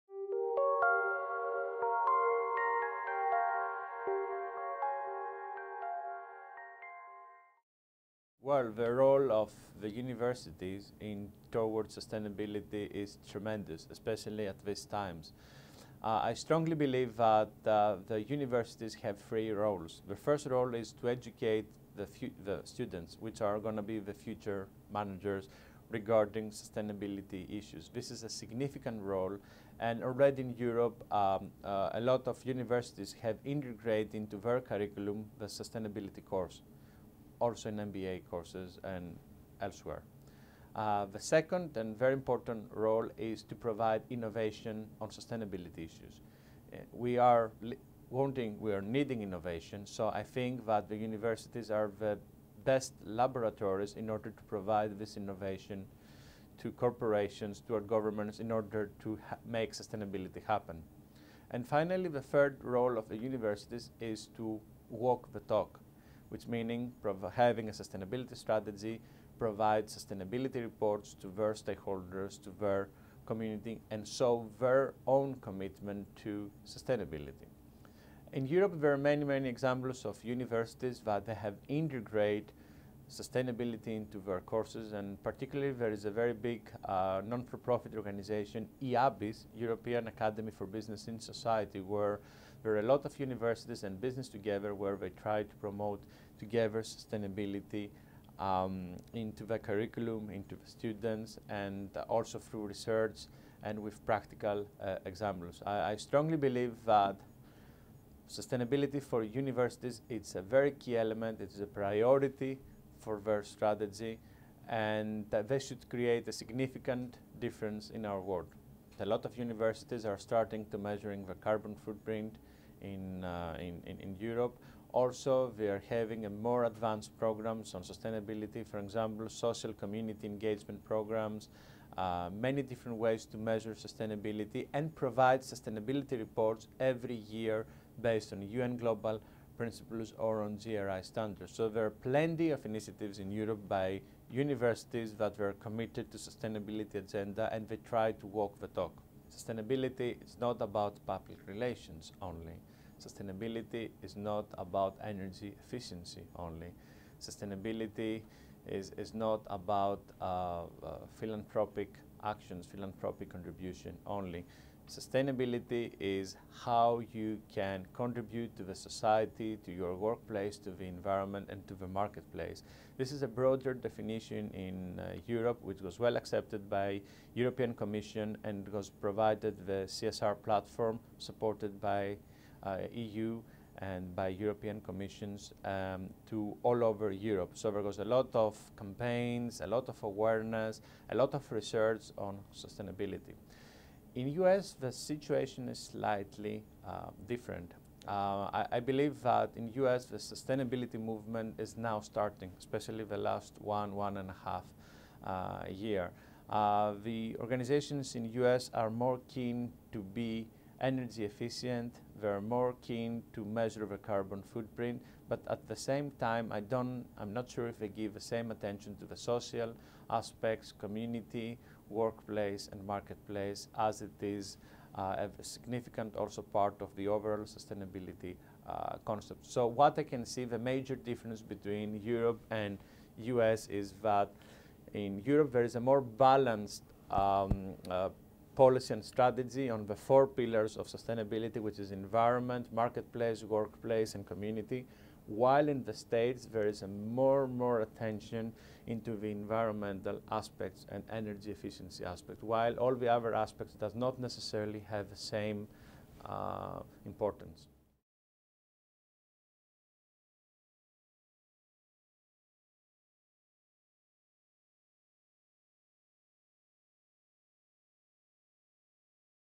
How do You Measure Sustainability?
Nikos Avlonas, one of the founders and Managing Director of the Centre for Sustainability and Excellence (CSE) shares insights on sustainability during his visit at DePaul University.